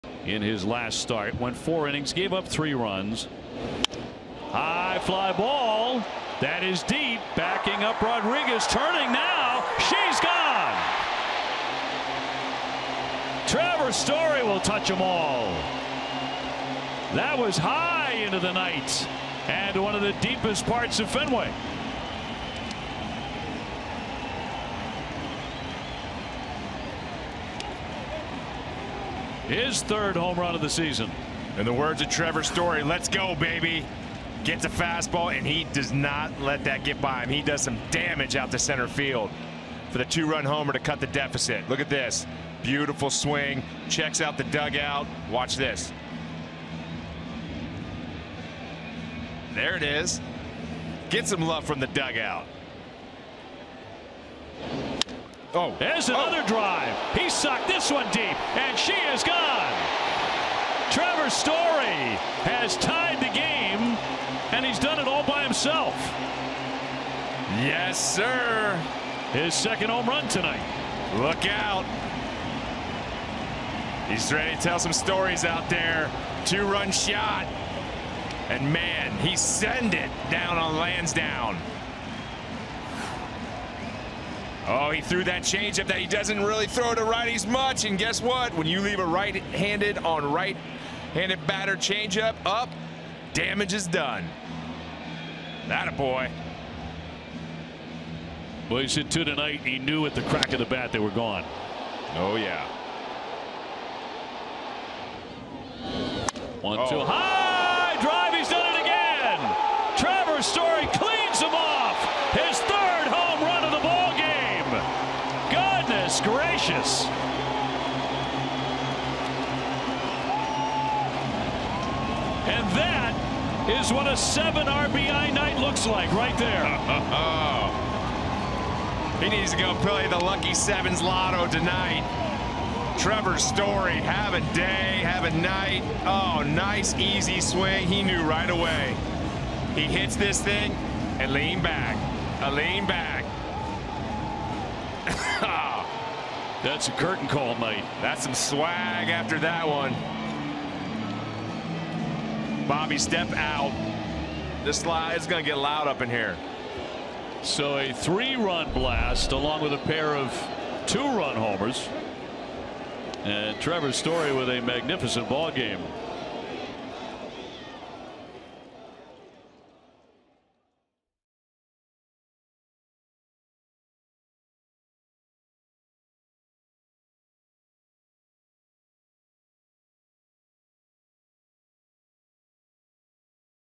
Trevor Story crushes THREE homers for the Red Sox!
Trevor Story WENT OFF for the Red Sox on Thursday night.